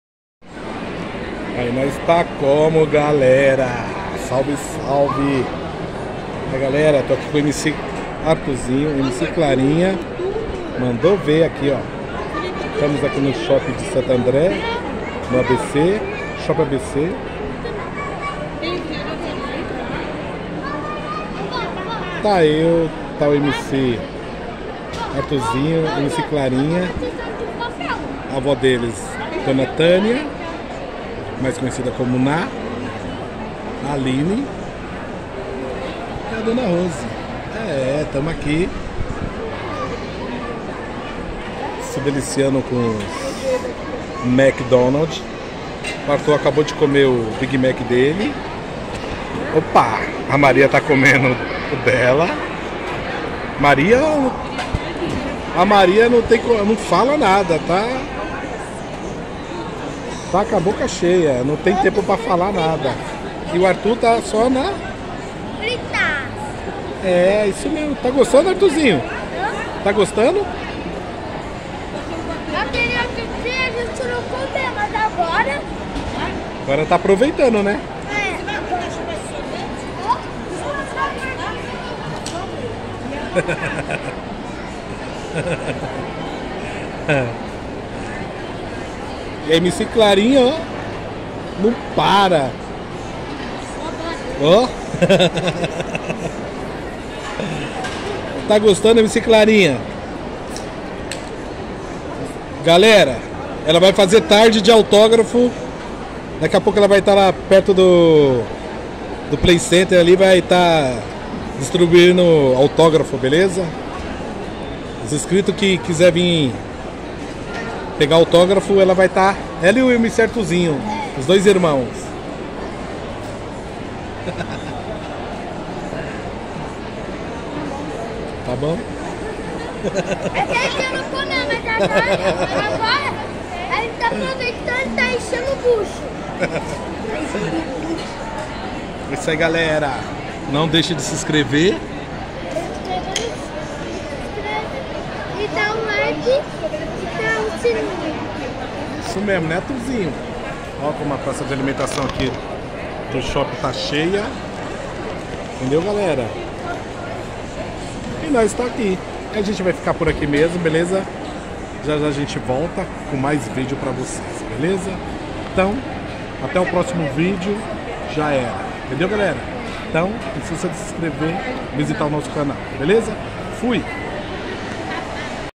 vlog : mc clarinha e mc arthuzinho feliz com os lanches do mc donalds,humm a melhor parte,é cumee😋👊🏻
instagram; @ marcelo araujo20182976
kwai:@marcelo araujo/os brothões oficial